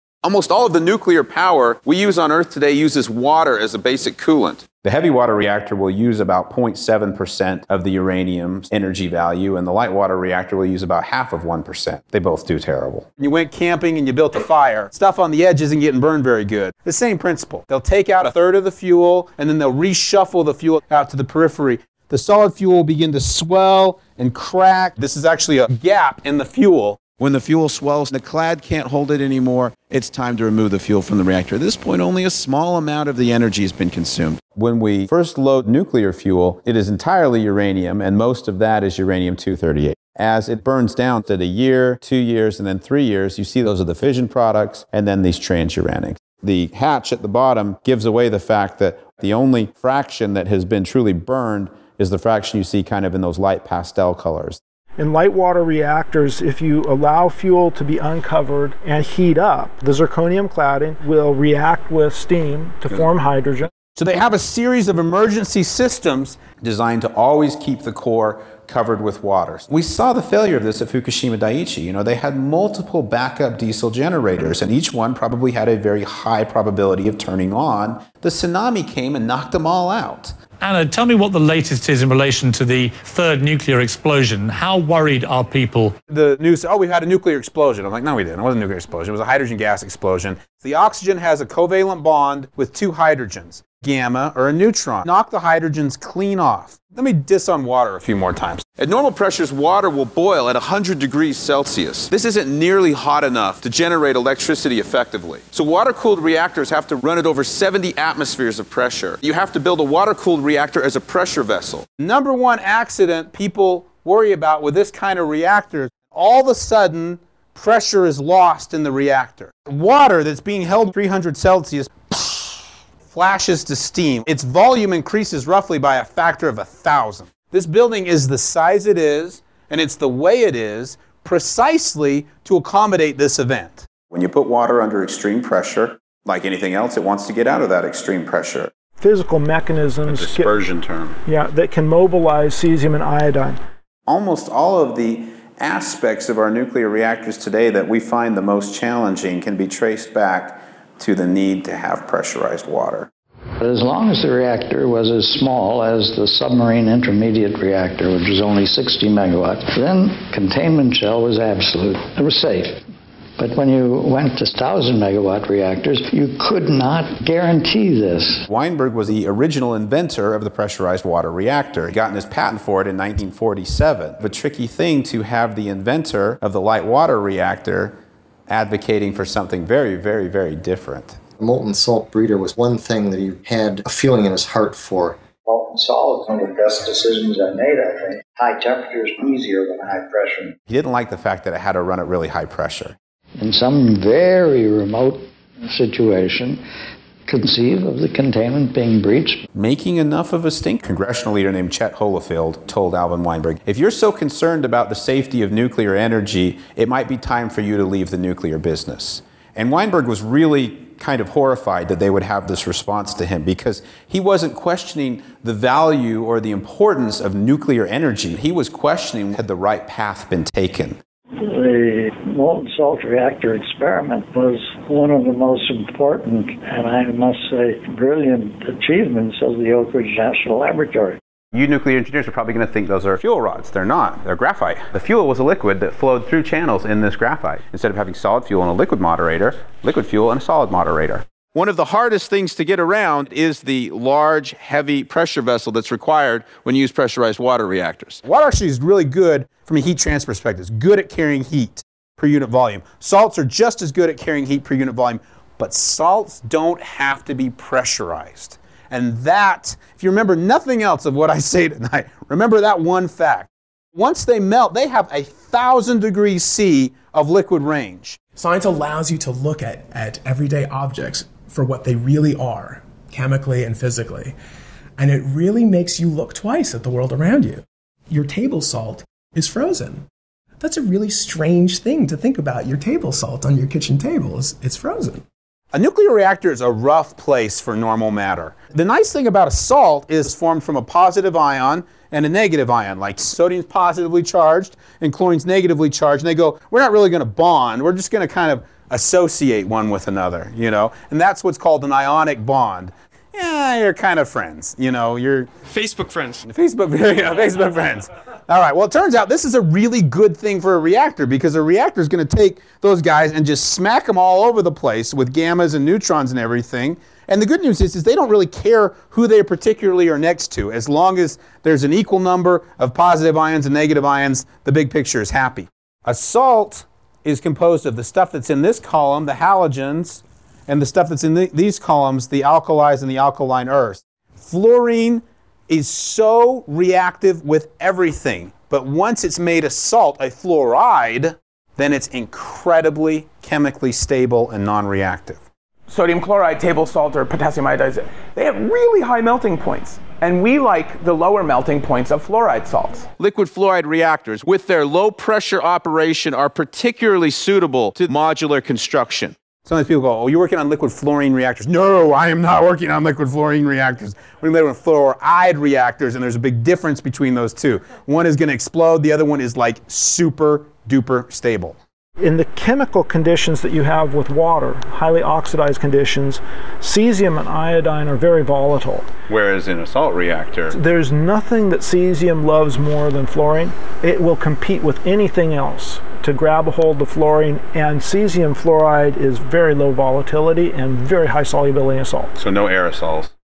Thorium 2017 - Part 1
Thorium & MSR & Nuclear assets used in Thorium Remix project.

Part 1 timecode range: 00m00s00f - 06m33s12f

Contents:
00:00 LWR: 0.05% of Uranium's Energy Potential
00:54 LWR: Chemical Instability
01:38 LWR: High Pressure
02:48 Alvin Weinberg
03:05 High Temperature not High Pressure
03:40 MSRE Achievement
03:51 Graphite
04:08 Move Heat No Pressure
04:45 Salt: Ionic Bonds
05:33 Fluoride vs Fluorine
06:16 Cesium & Iodine

This video belongs to a 10-part playlist representation of that "THORIUM 2017" 34 minute video.